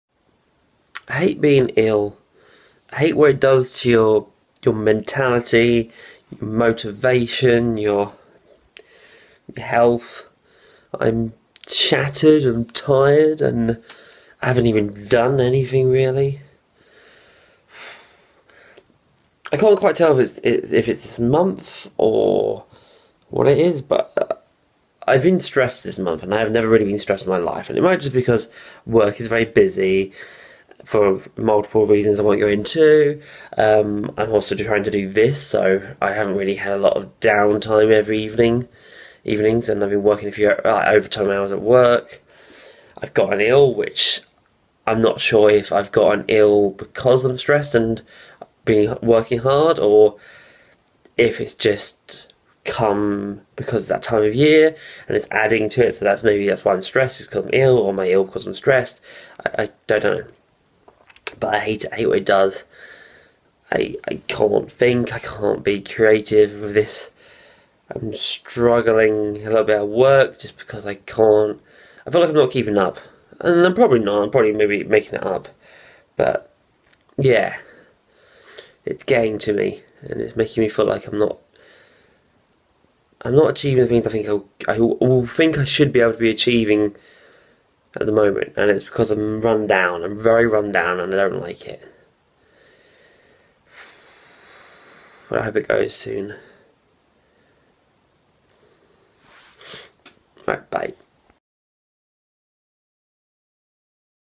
vedj day 27 - feeling run down
not that I need to tell anyway, but being ill really sucks!